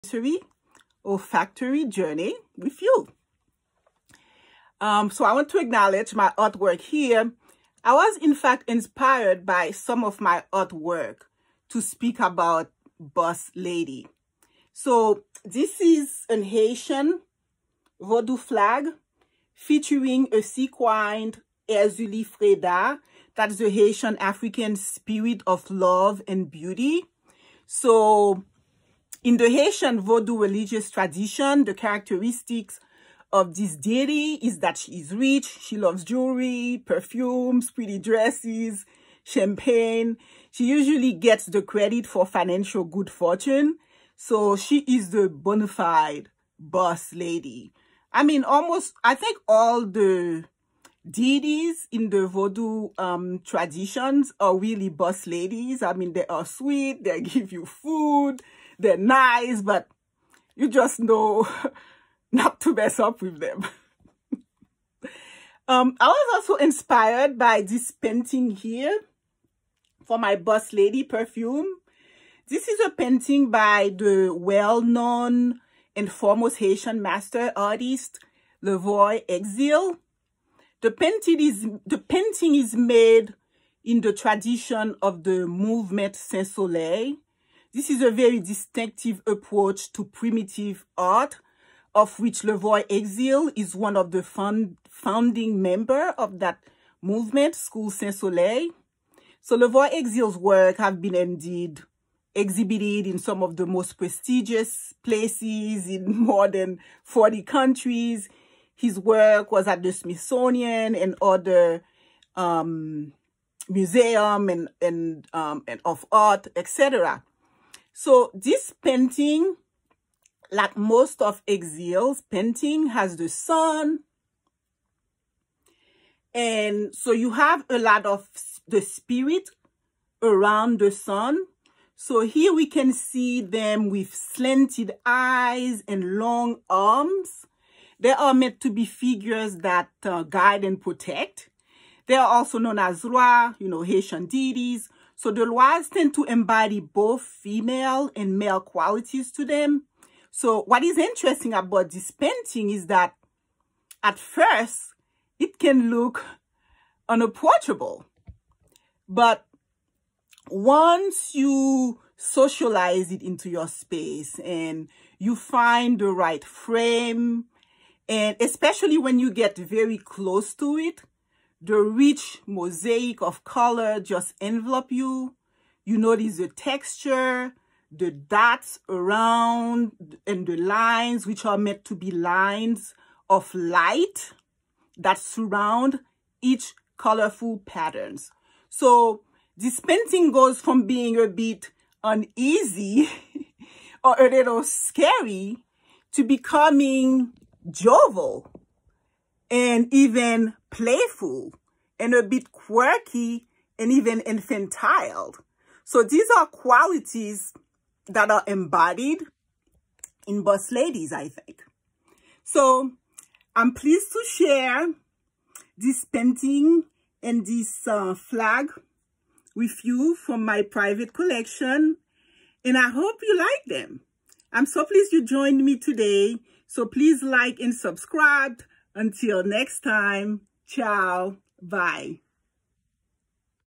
Haitian Art Highlight: Levoy Exil Saint Soleil Style Painting
My Review of Celebrated Sequined Haitian Flag Art Style and a Levoy Exil Painting